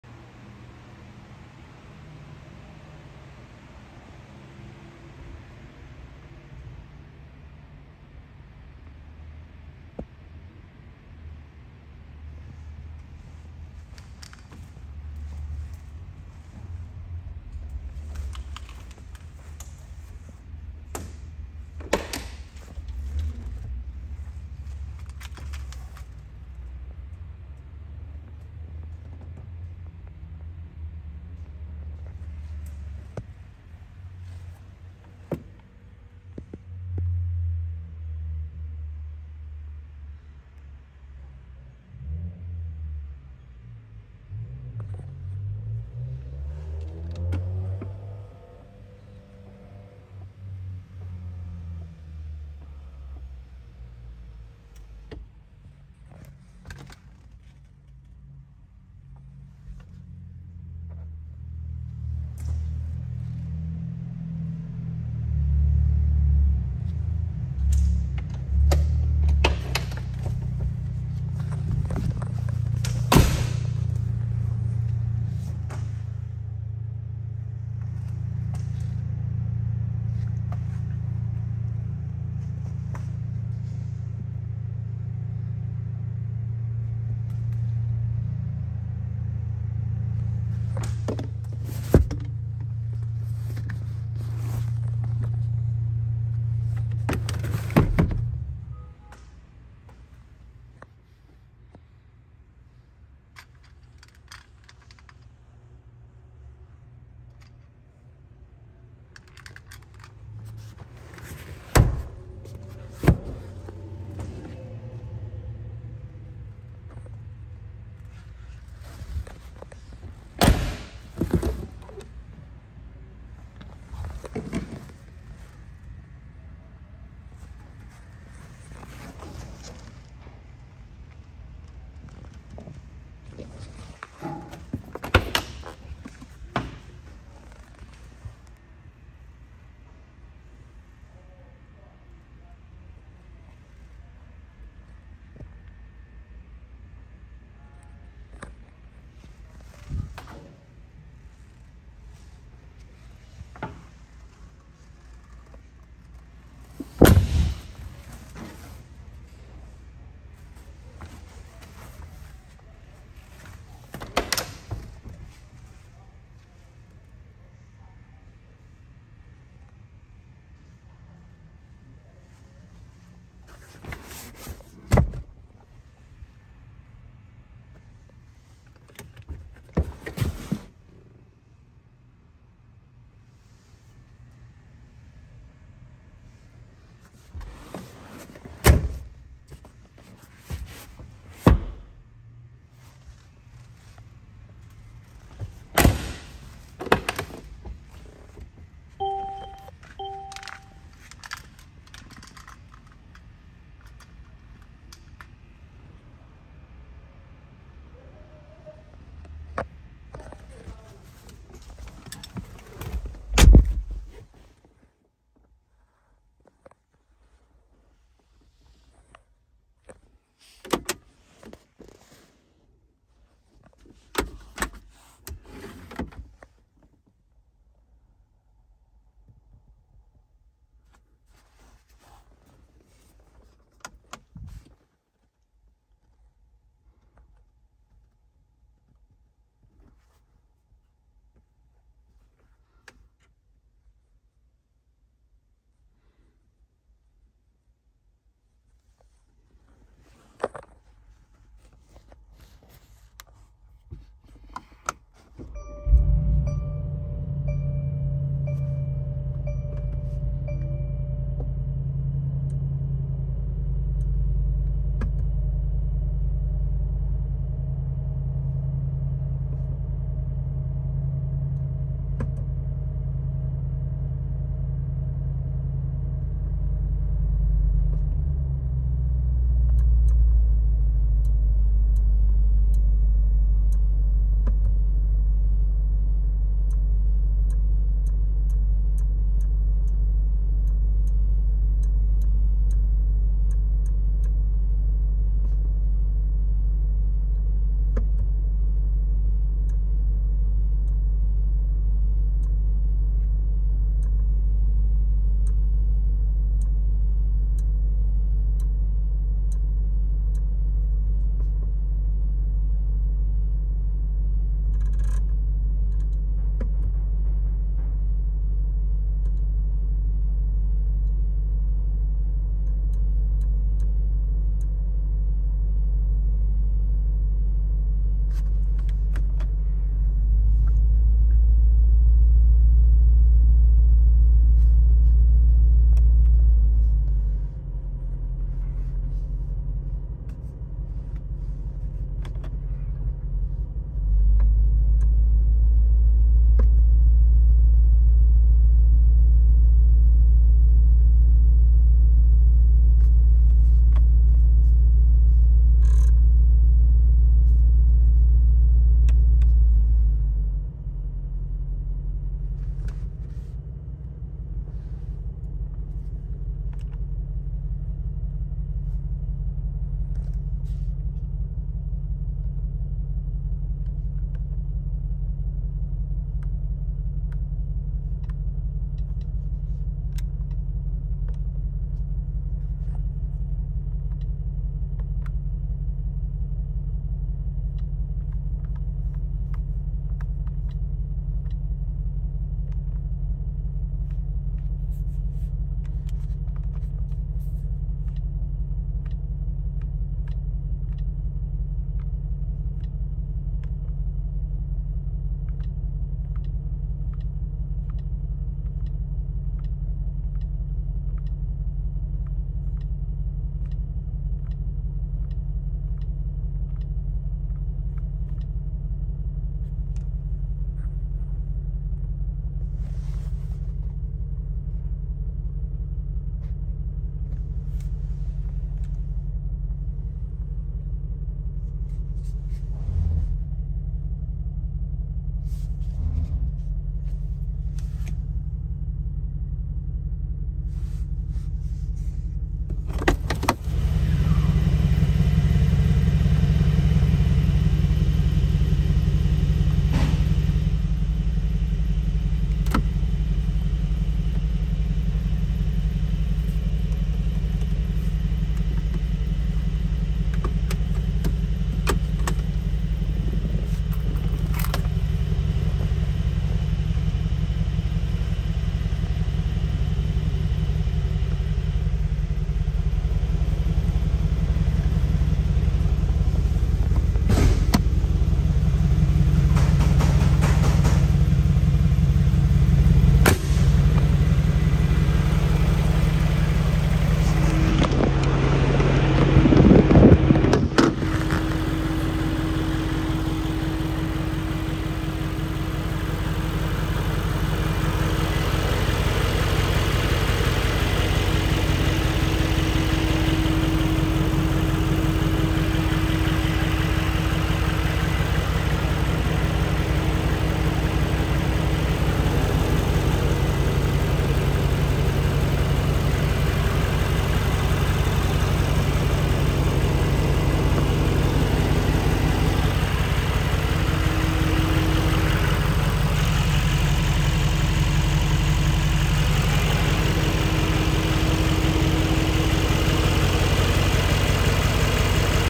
2022 Mercedes Benz AMG G-Class G63 - 577 HP 4.0L V8 Twin Turbo - G manufaktur China Blue - For Sale
LOCATED IN GALLERIA AREA IN HOUSTON, TEXAS, USA. OVER 300 CARS